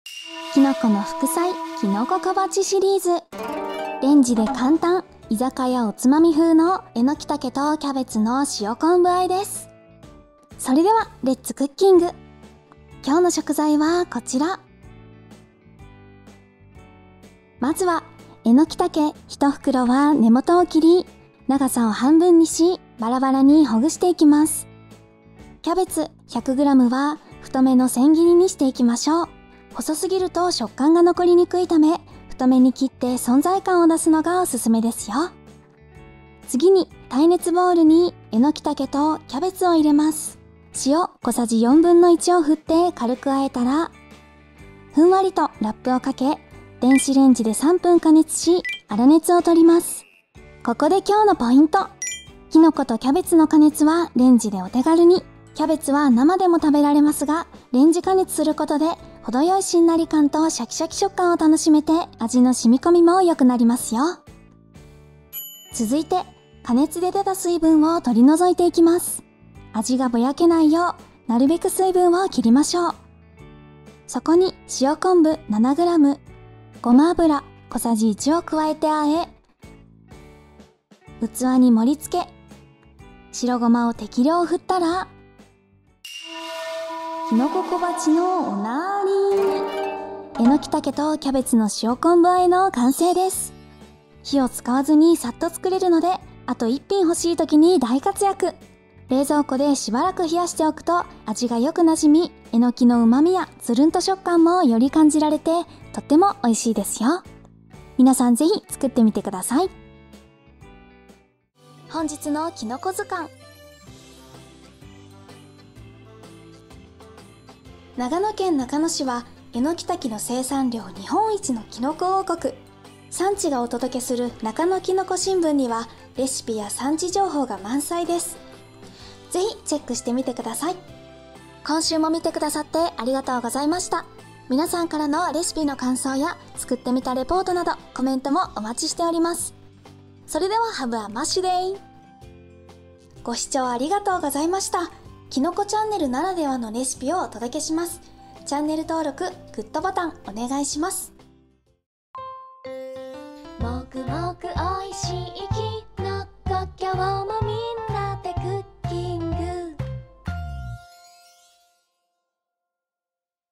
［居酒屋風きのこ小鉢］えのきたけとキャベツの塩昆布和え
居酒屋のおつまみ風、レンジで作れるお手軽副菜です。つるんと食感のえのきにシャキシャキのキャベツが好相性！塩昆布とえのきのうま味に、キャベツの甘みがあとを引くおいしさです。

えのきたけ 1袋（200g）
塩昆布 7g
ごま油 小さじ1
白ごま 適量

▼簡単・お手軽「居酒屋風きのこ小鉢」▼

◇イラスト・声：リベットボタン

◆きのこのお役立ち情報を発信中◆

きのこレシピ専門サイト「なかのきのこレシピ」